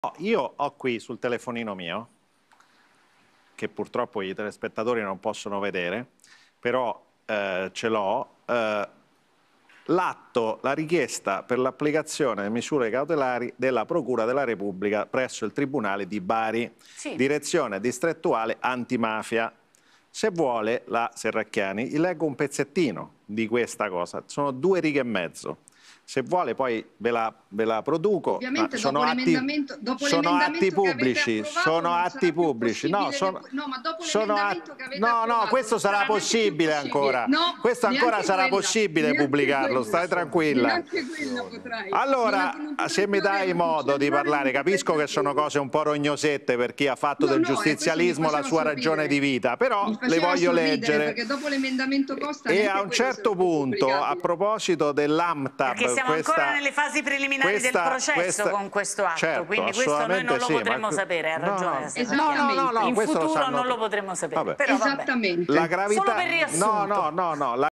Decaro, Serracchiani a Nevi: "Leggi cose che non potremmo pubblicare"
Bagarre in studio fra Debora Serracchiani (Pd) e Raffaele Nevi (Forza Italia)